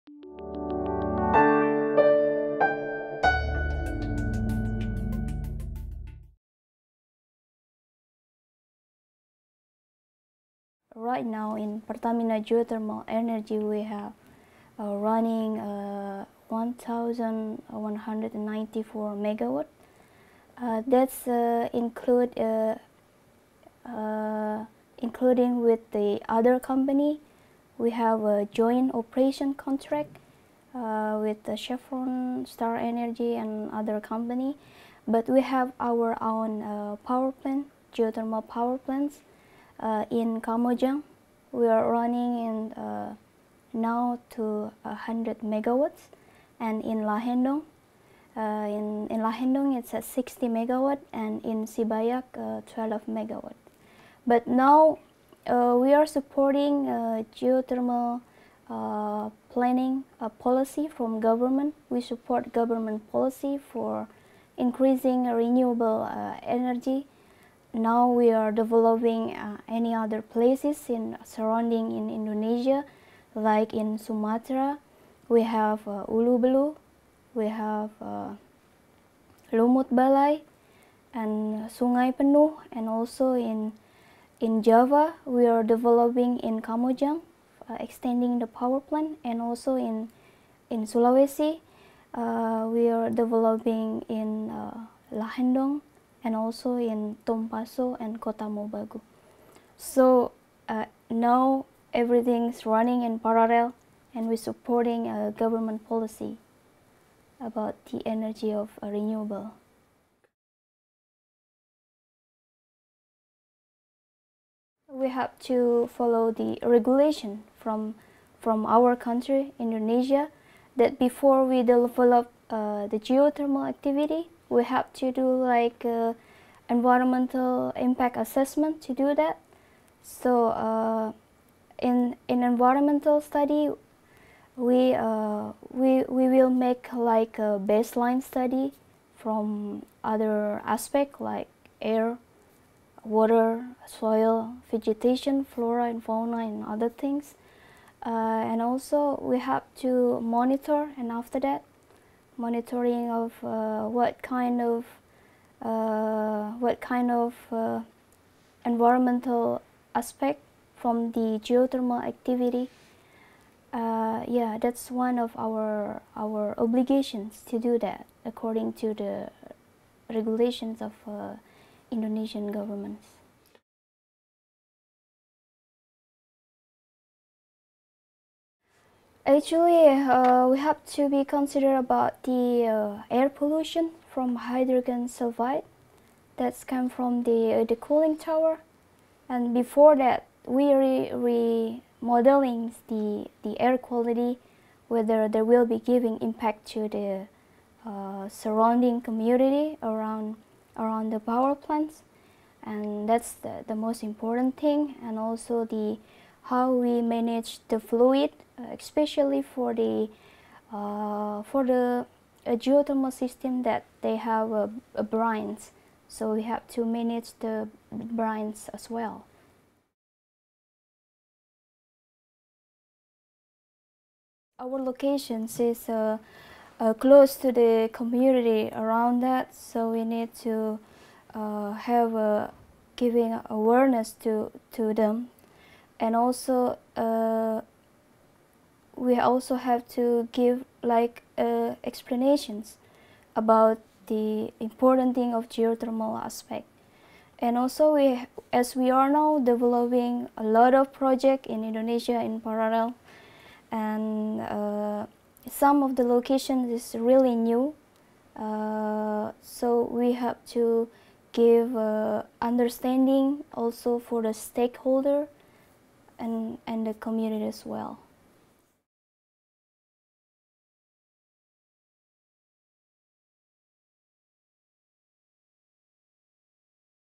Geothermal energy in Indonesia
UN University Training Programme fellow Irma Khoirunissa shares his views on geothermal energy developments in Indonesia.